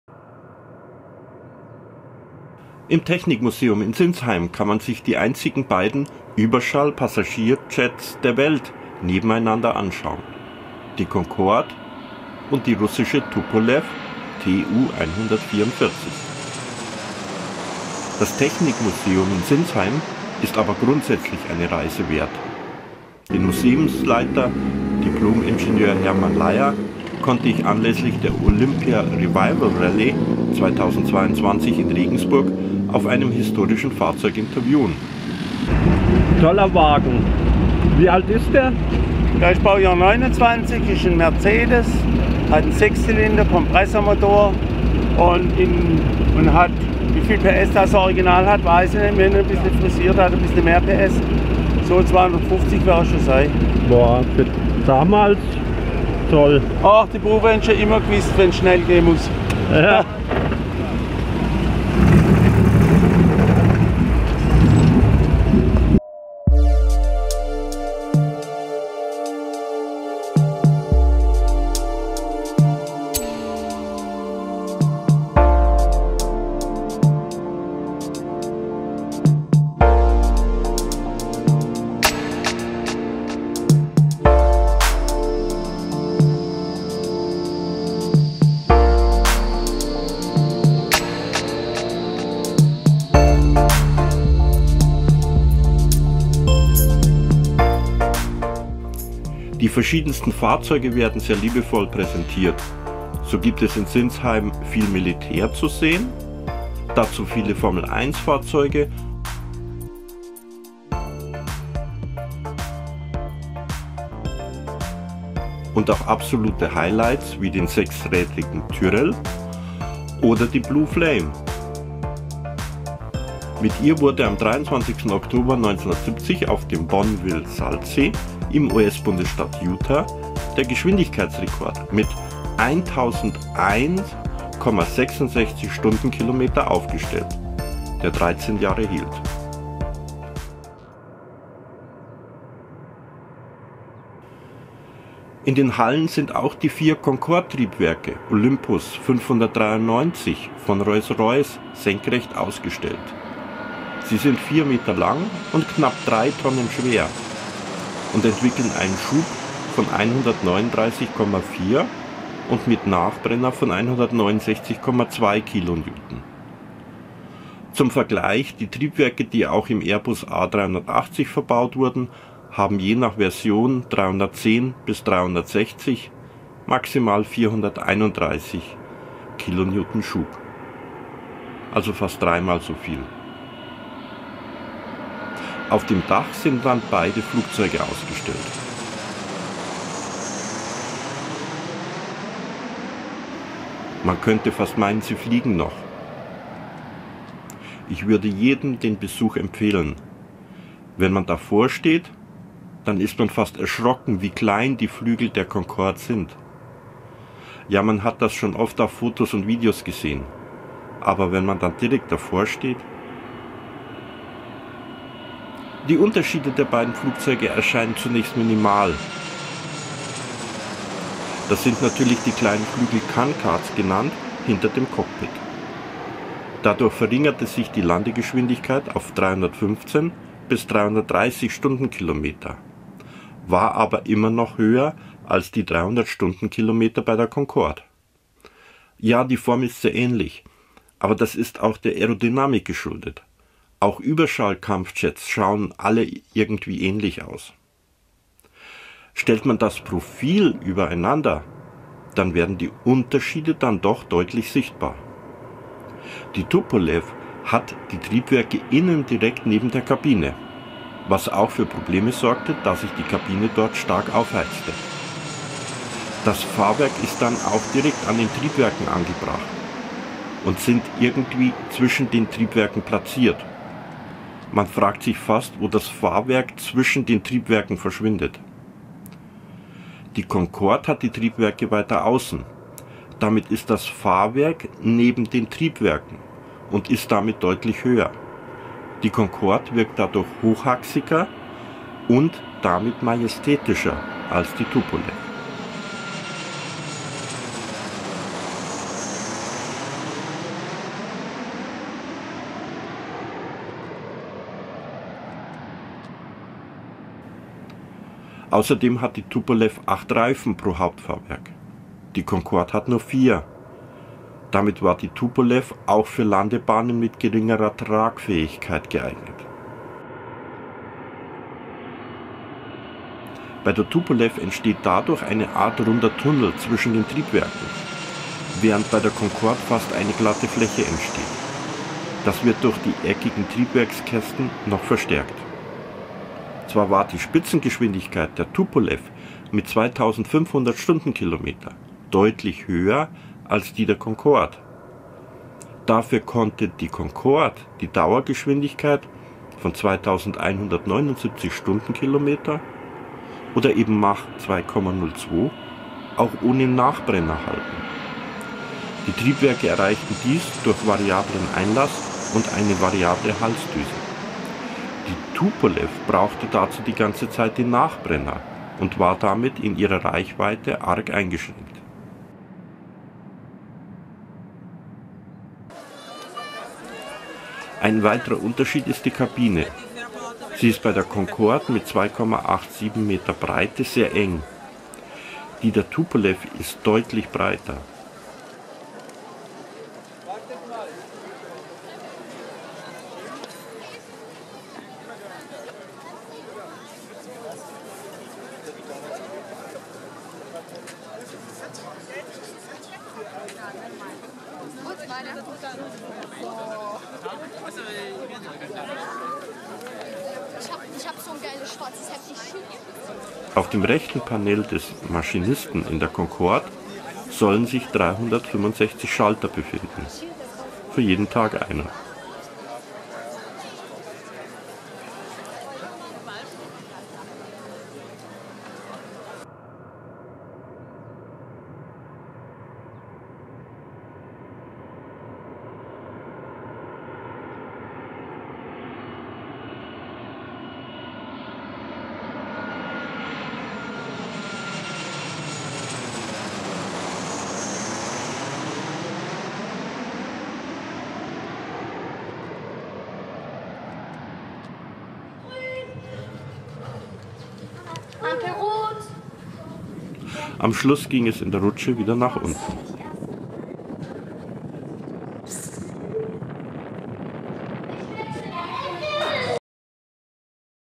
Concorde vs Tupolev TU-144
Bei einem Besuch im Technikmuseum Sinsheim werden die Überschallpassagierjets  Concorde mit der russischen Tupolev TU-144 miteinander vergleichen. Dabei werden durchaus Unterschiede sichtbar. So die Canards, die kleinen Flügel hinter dem Cockpit bei der Tupolev. Die Tupolev hat aber auch die Triebwerke direkt innen an der Kabine, während sie bei der Concorde deutlich weiter außen sind. Auch das Fahrwerk istd adurch anders.

0:17 Technikmuseum
2:20 Triebwerke
3:02 Canards
4:07 Profil
6:15 Kabine
7:09 Ende